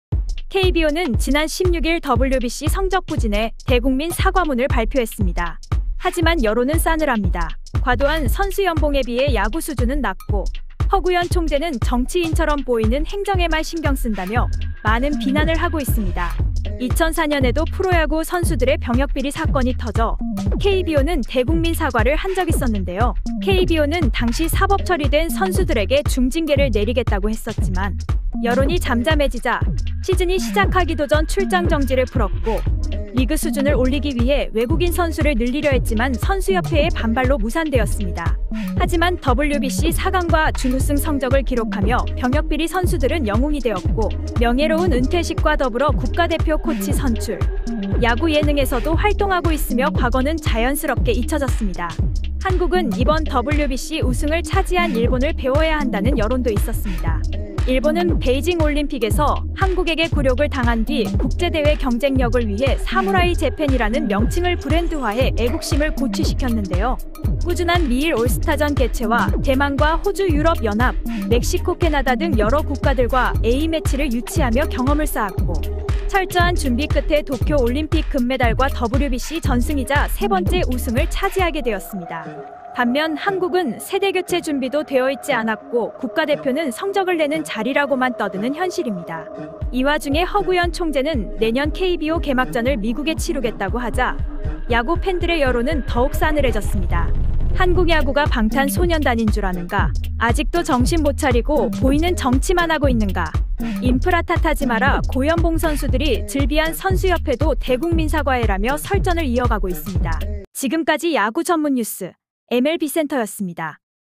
KBO, WBC 참패에 대국민 사과 "추신수가 옳았다" 여론 확산
허구연 총재가 WBC 야구 대표팀 성적 부진에 대국민 사과를 했고 추신수의 말이 옳았다는 의견들이 나오고 있습니다